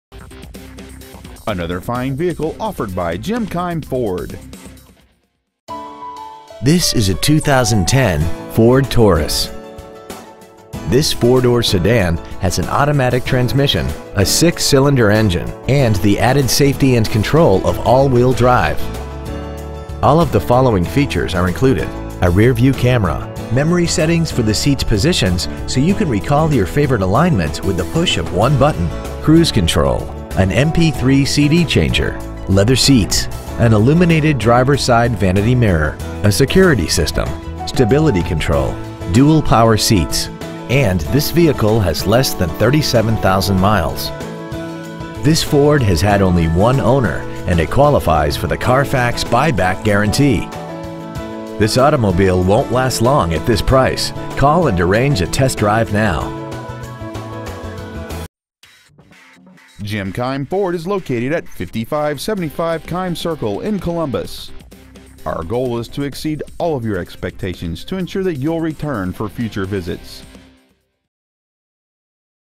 2010 Ford Taurus Columbus OH
Year : 2010
 Make : Ford
 Model : Taurus
 Engine : 3.5L 6-Cylinder
 Trans . : automatic
 Exterior : Ingot Silver Metallic
 Miles : 36,723
 Interior : Charcoal black

 Preowned 2010 Ford Taurus Columbus OH